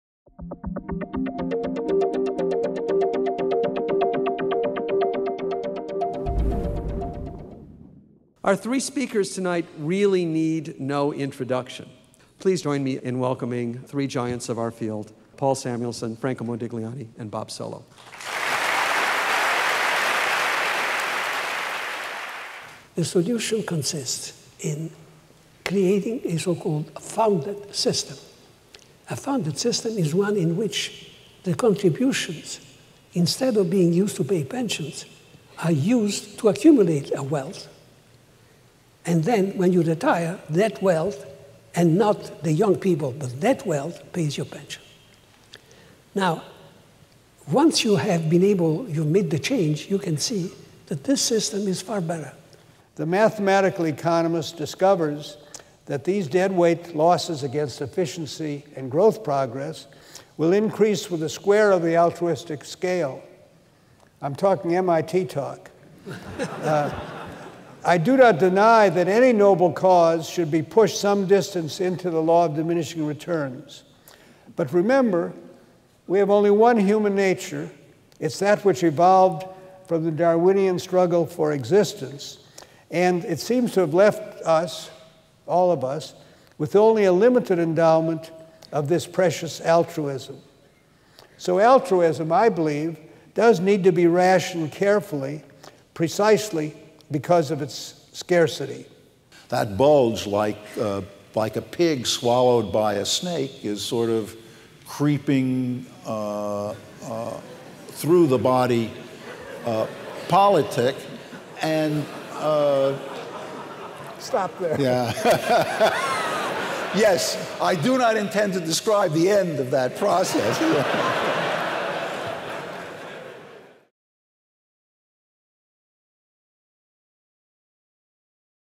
Nobel Prize-Winning Economists
This video captured a rare appearance of MIT's Nobel Prize-winning economists gathered to deliver the inaugural Ford/MIT Nobel Laureate Lecture on September 18, 2000 in Kresge Auditorium. Introduced by Chancellor Lawrence S. Bacow, the distinguished economists included Professors Emeriti Paul A. Samuelson, Franco Modigliani, and Robert M. Solow. Their lecture was titled "The US Economy: The Last 50 Years and the Next 50 Years."